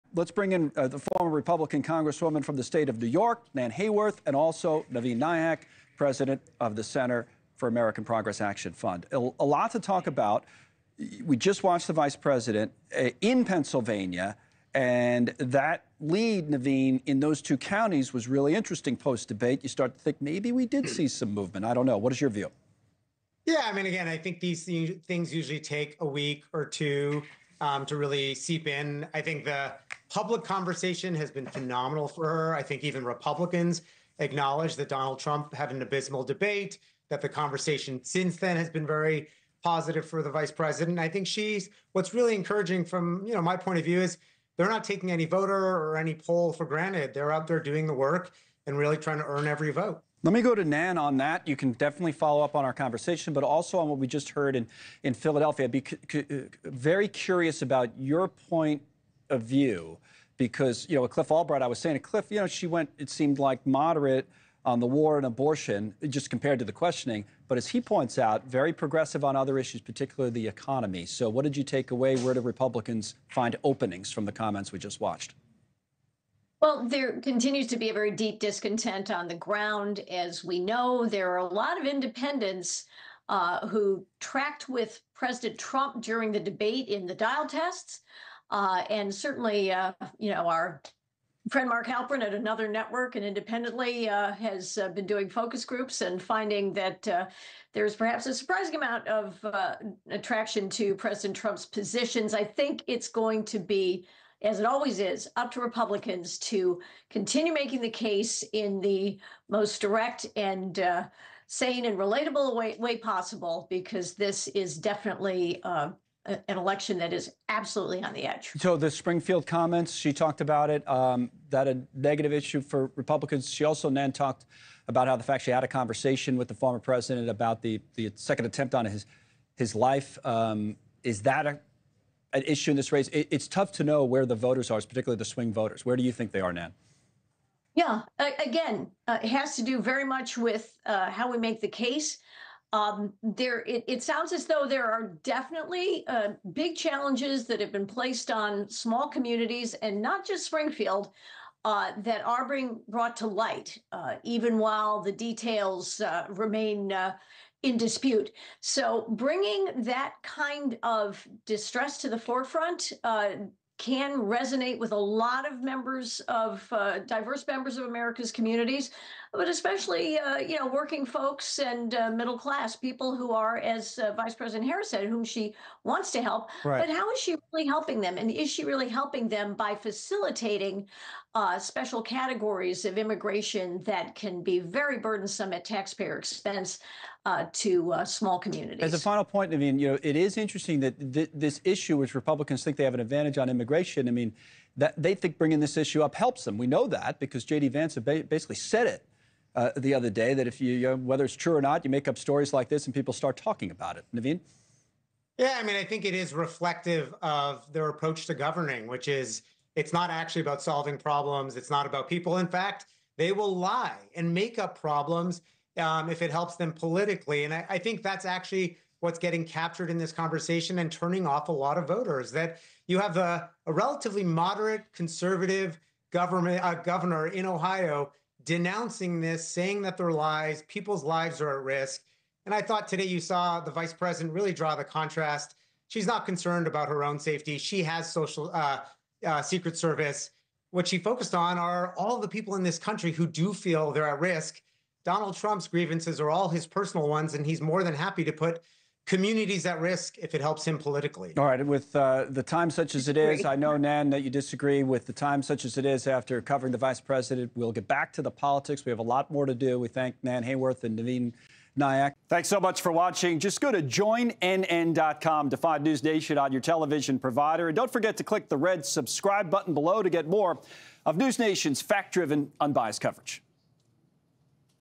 Harris has six-point lead over Trump after debate: Poll | NewsNation Now
Postdebate polling done by Morning Consult shows Vice President Kamala Harris in front of former President Donald Trump by 6 points, the biggest lead to date for the Democratic nominee.
Navin Nayak, president of the Center for American Progress Action Fund, said this is because even Republicans acknowledged Trump had an "abysmal" debate.
Former New York Rep. Nan Hayworth, a Republican, says there are still independents who were not convinced by the debate and are leaning toward Trump.